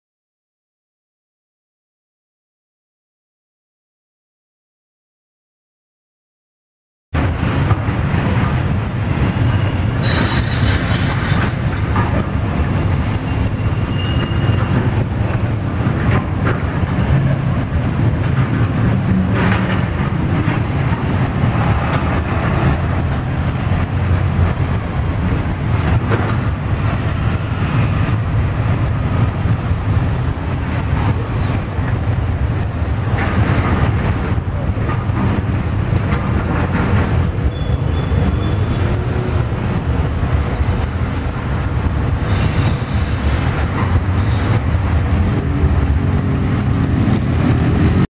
Motoqueiro avança Farol vermelho e atinge outra moto
Foi no cruzamento da Avenida Anhaia Mello com a Rua Américo Vespucci por volta das 8:00 deste dia 23/04/2014.